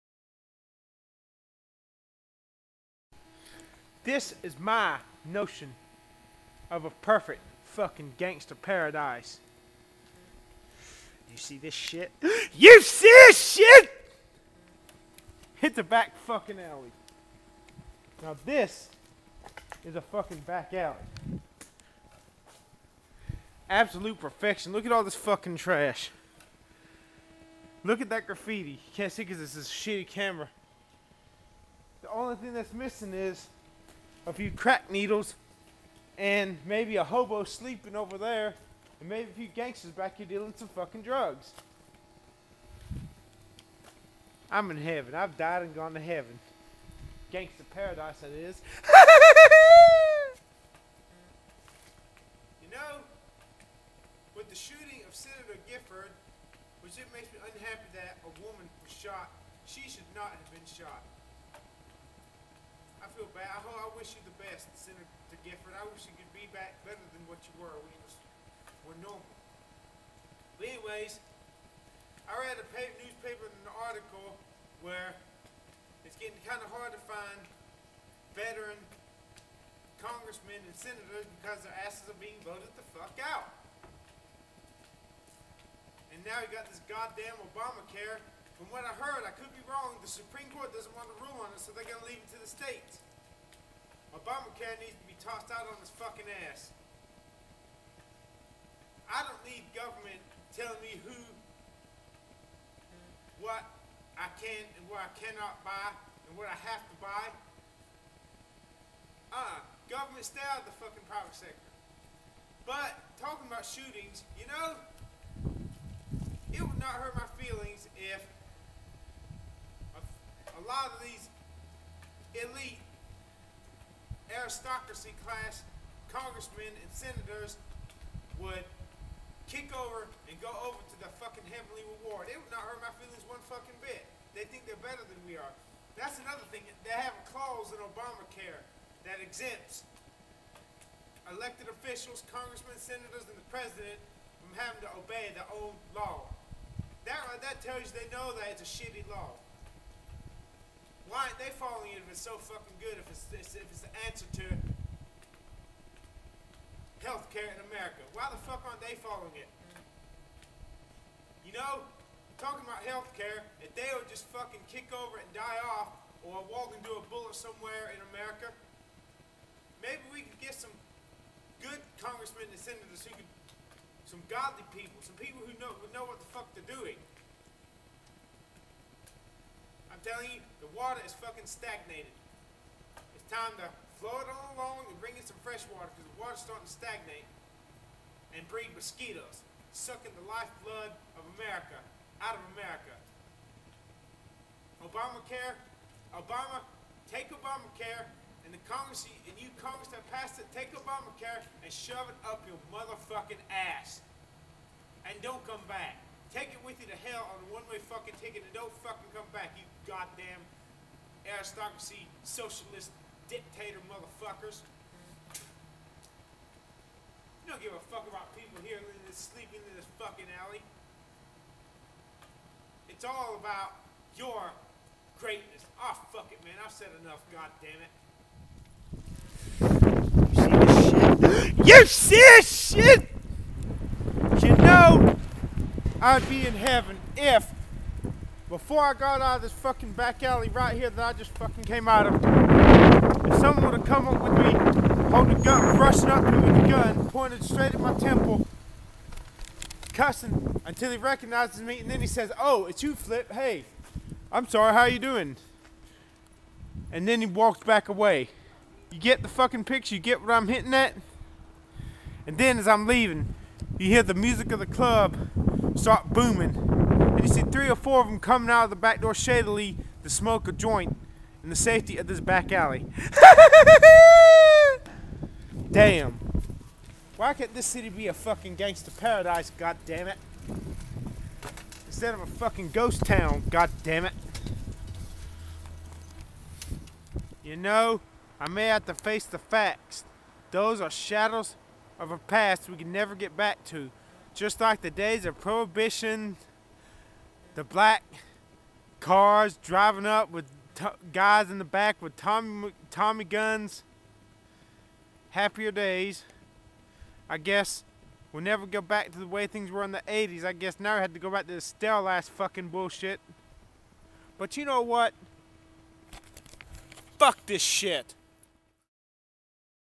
Diatribe in the DARK SPOOKY ALLEY tonight, about government and politics, and Obamacare!!!
THIS IS MY notion of a perfect fucking gangsta paradise (in a back alley), full of trash and some GRAFFITI on the walls!
Senator Gifford had just been shot...

The fact the congress exempted itself from the Obamacare shows that it's a shitty law.